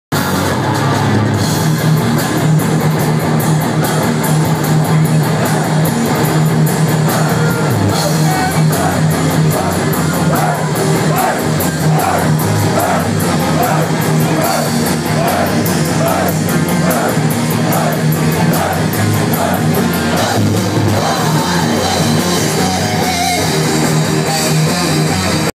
Metalfest Croatia 2012 - Tomas 'Elof' Elofsson - Hypocrisy
Tomas 'Elof' Elofsson from Hypocrisy play "Thomas Guitars"